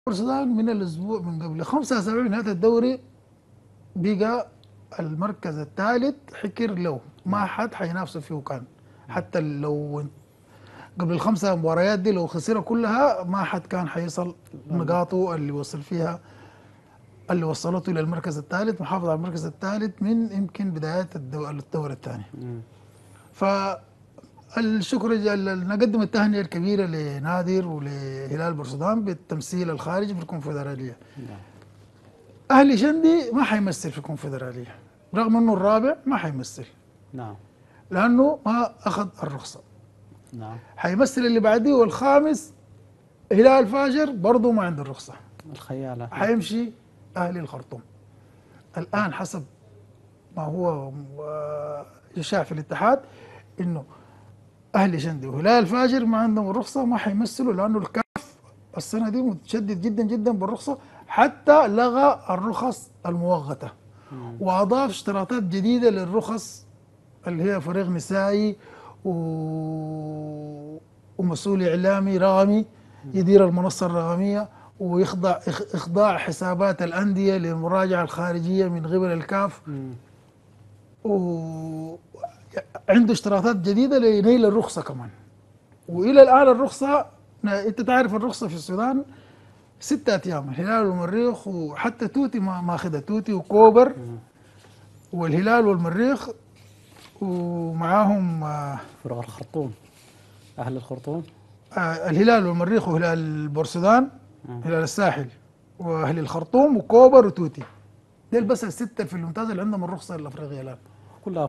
الأهلي شندي وهلال الفاشر لن يمثلا السودان في الكونفدرالية لعدم حصولها على الرخصة الأفريقية
🗣️ أ/ الفاضل حسن
محلل رياضي